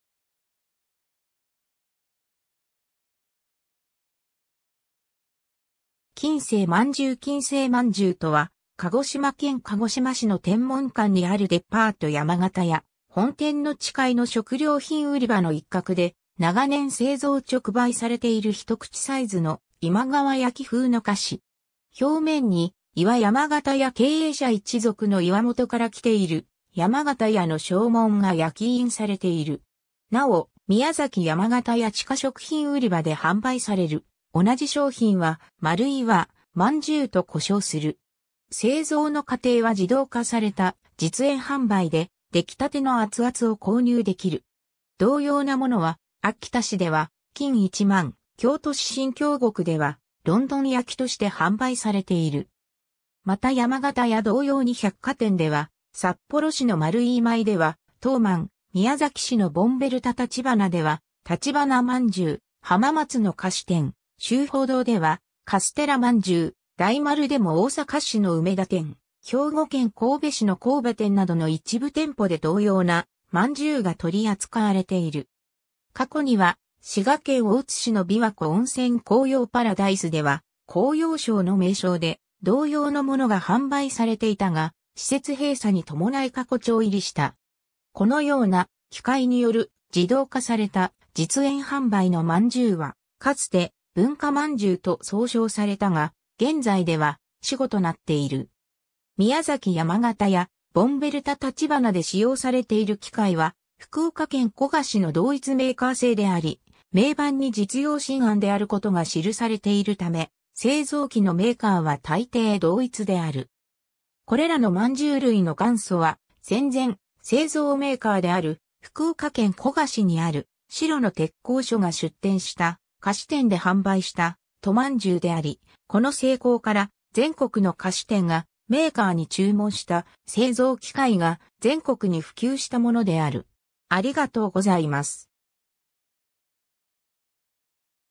金生饅頭
金生饅頭 金生饅頭（きんせいまんじゅう）とは、鹿児島県鹿児島市の天文館（金生町）にあるデパート山形屋本店の地階の食料品売り場の一角で長年製造直売されている一口サイズの今川焼風の菓子。
 表面に『○岩⇒山形屋経営者一族の岩元からきている山形屋の商紋』が焼印されている。
なお、宮崎山形屋地下食品売り場で販売される同じ商品は『まるいわまんじゅう』と呼称する。
製造の過程は自動化された実演販売で、出来立ての熱々を購入できる。
同様なものは秋田市では金萬、京都市新京極ではロンドン焼として販売されている。
 また山形屋同様に百貨店では、札幌市の丸井今井では『とうまん』、宮崎市のボンベルタ橘では『橘まんじゅう』、浜松の菓子店「秋芳洞」では『カステラ饅頭』、大丸でも大阪市の梅田店（JR大阪駅ビル）、兵庫県神戸市の神戸店などの一部店舗で同様な饅頭が取り扱われている。
過去には滋賀県大津市のびわ湖温泉紅葉パラダイスでは『紅葉焼』の名称で同様の物が販売されていたが施設閉鎖に伴い過去帳入りした。
 このような、機械による自動化された実演販売の饅頭は、かつて『文化饅頭』と総称されたが、現在では死語となっている。
宮崎山形屋、ボンベルタ橘で使用されている機械は福岡県古賀市の同一メーカー製であり、銘板に実用新案であることが記されている為、製造機のメーカーはたいてい同一（キノ式）である。
 これらの饅頭類の元祖は、戦前、製造メーカーである福岡県古賀市にある城野鉄工所（現在は機械製造のみ）が出店した菓子店で販売した『都まんじゅう』であり、この成功から全国の菓子店がメーカーに注文した製造機械が全国に普及したものである。